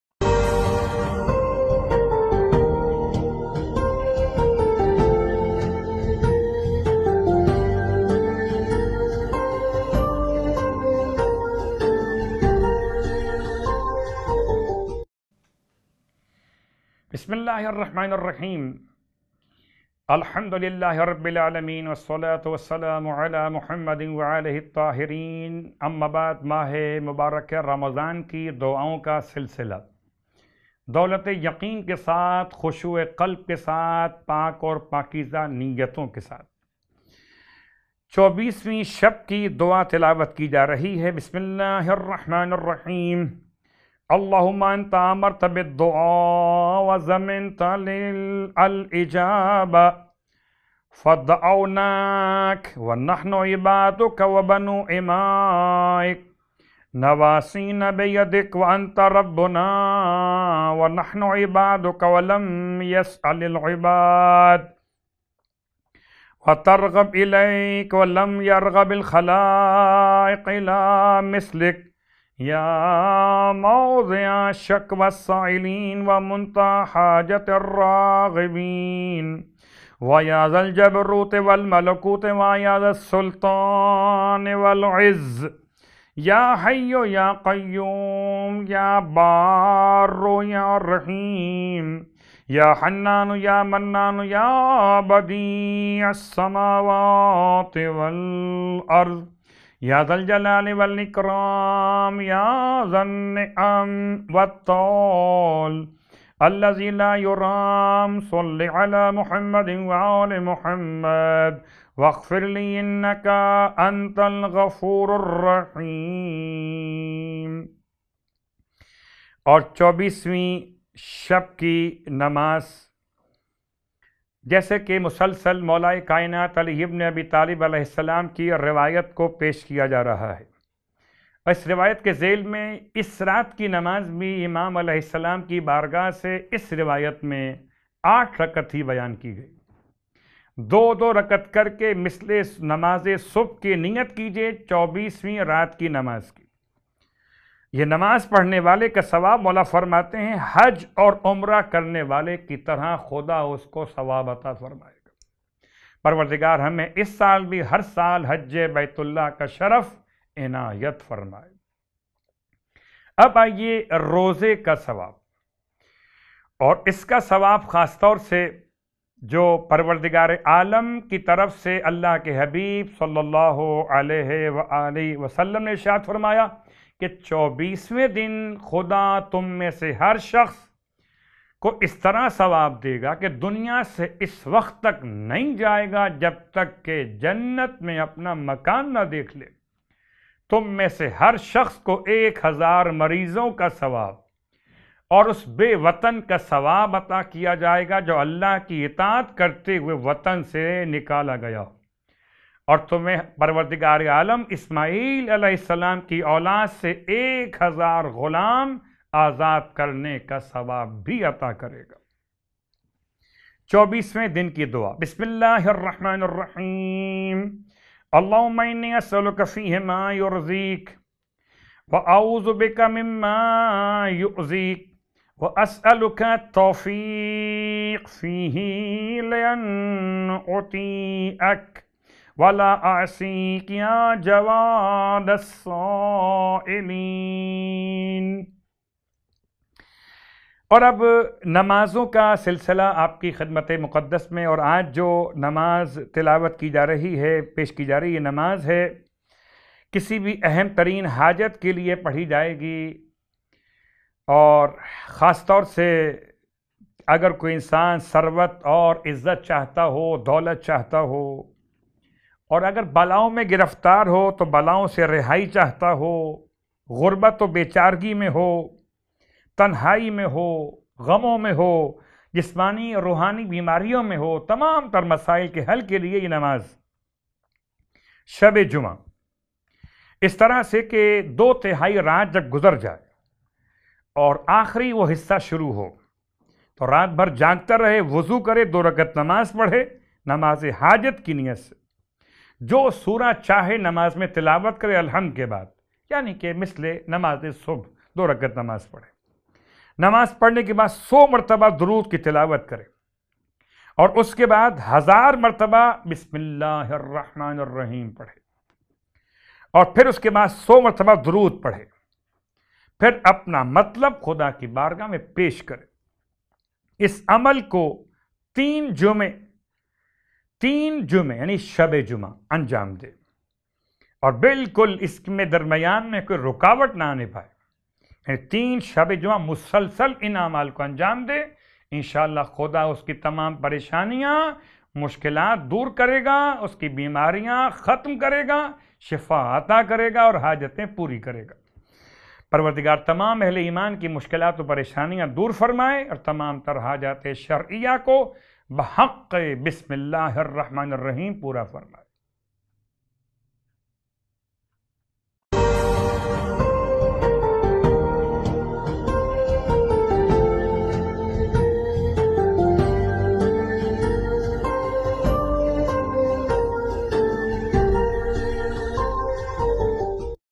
24th Ramadan Episode | Amaal e Ramadan | Maulana Syed Mohammad Ali Naqvi
Date: 24th Ramadan
Scholar: Maulana Syed Mohammad Ali Naqvi
Production: Knowledge Gateway Productions